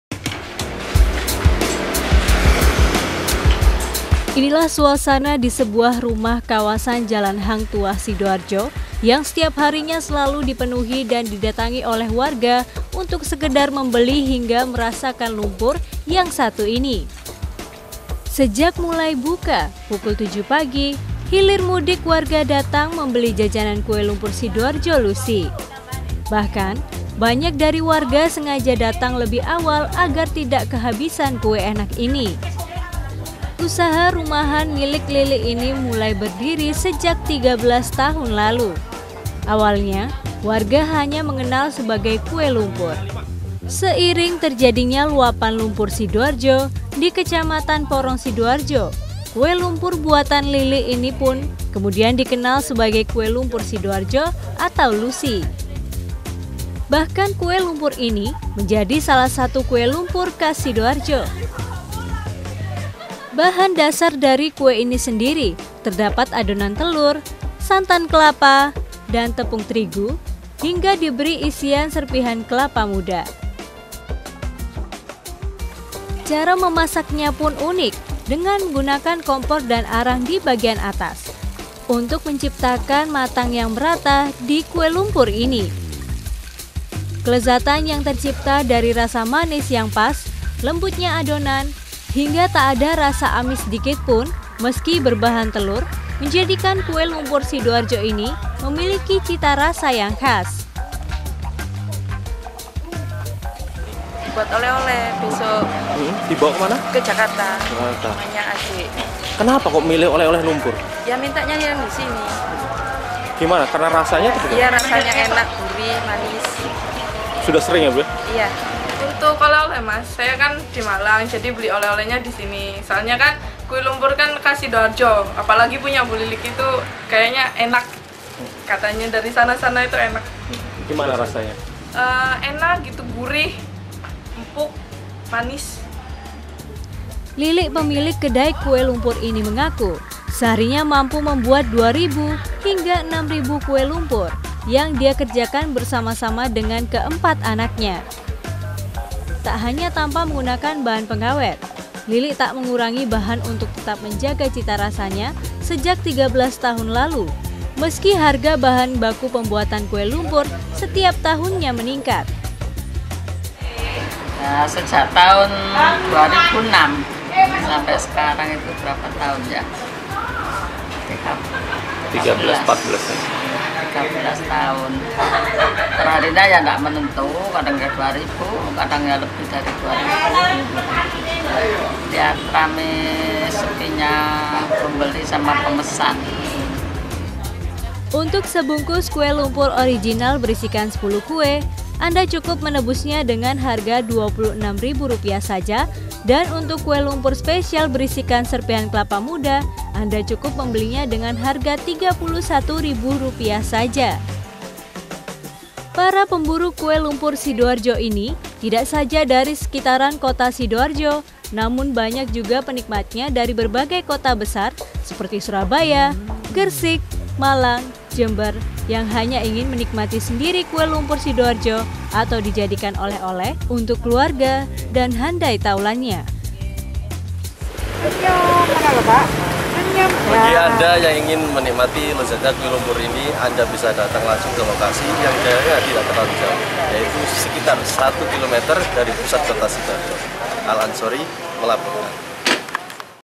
SIDOARJO - Kue Lumpur Semakin Melejit
Sajian kue tentunya tidak asing lagi bagi pecinta kuliner. Di sidoarjo terdapat kue lumpur yang sudah belasan tahun tetap eksis diburu pecintanya. Keberadaan semburan Lumpur  Sidoarjo menjadi kue lumpur ini dikenal sebagai kue lumpur Sidoarjo atau lusi. Banyak dari warga baik itu dari Sidoarjo maupun dari  luar kota berburu jajanan ini.